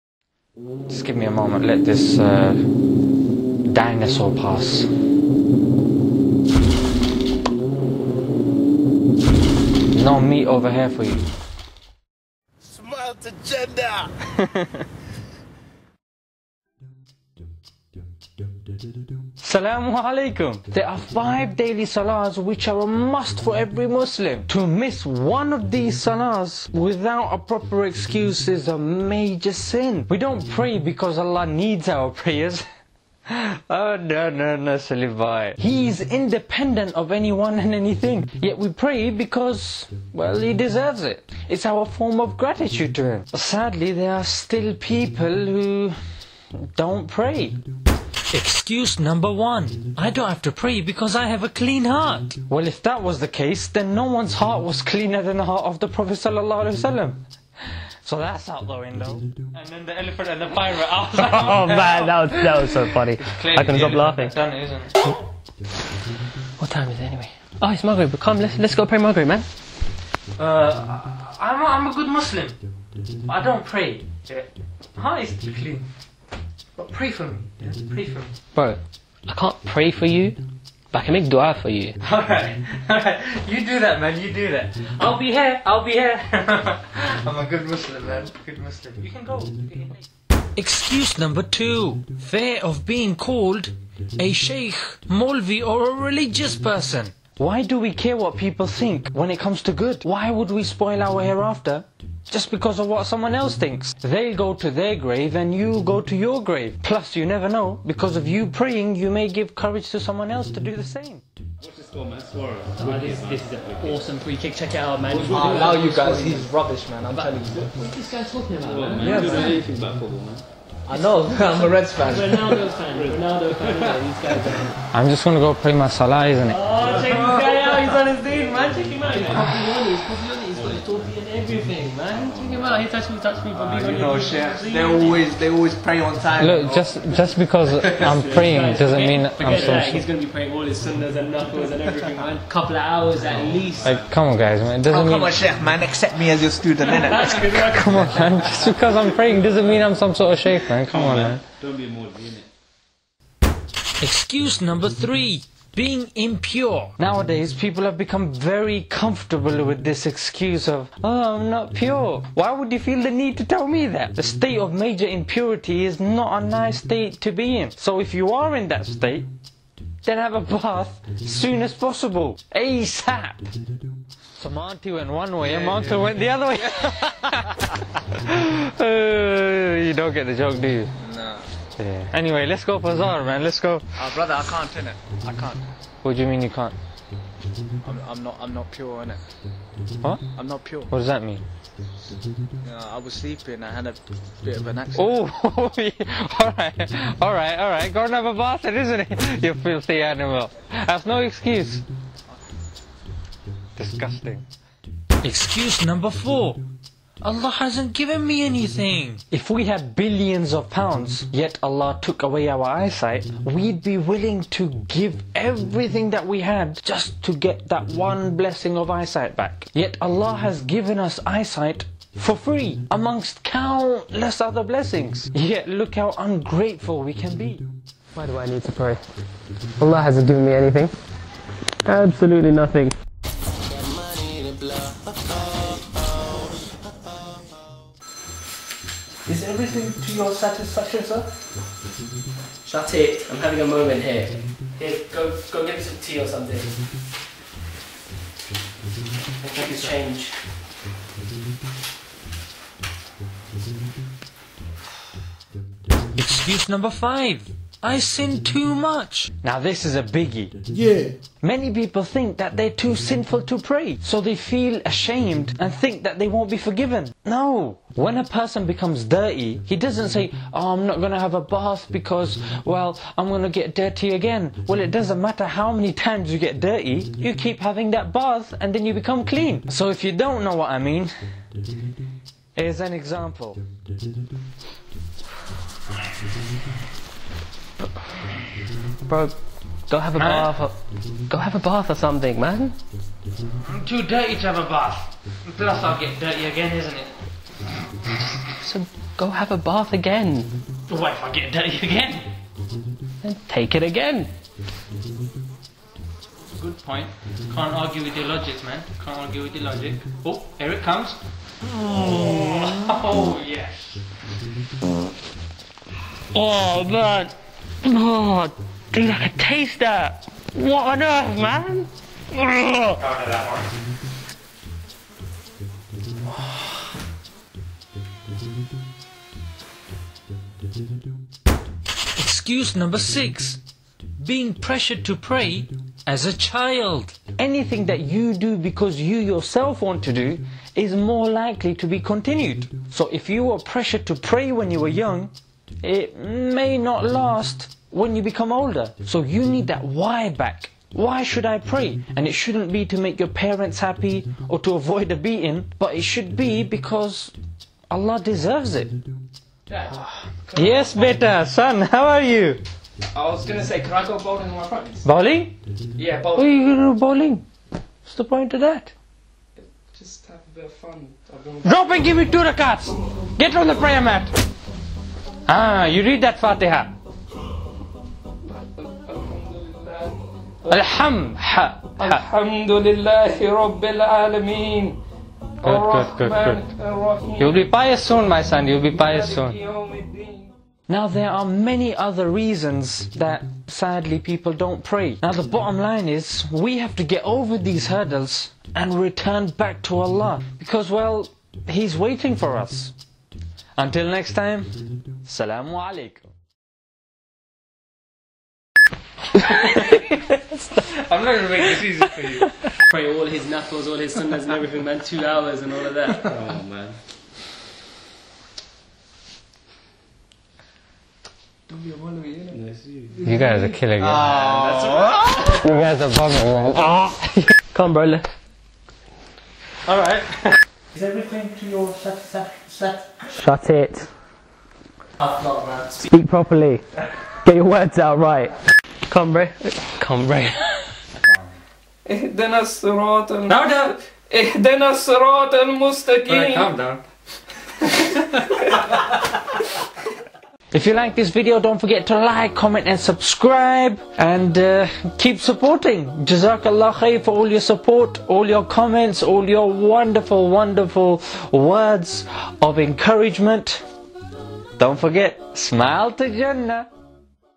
TYPES OF PEOPLE WHO DON'T PRAY
Why People Don't Pray
(Click captions to enable subtitles)

Salaah, if it is offered with a sincere heart, proper devotion and mental concentration, is particularly vested with the property of cleansing the heart, reforming one's life and ridding it of sins and impurities. It generates love, piety and also truth. It promotes fear of Allah in man.

Here are some excuses we cover in this video, of why people don't pray:
1) I don't have to pray because I have a clean heart.
2) Fear of being called a Shaykh/Molvi/religious
3) Being impure
4) Allah hasn't given me anything.
5) I sin too much.
6) Being pressured to pray as a child.

It will help us a great deal. Jazakallahu khairun.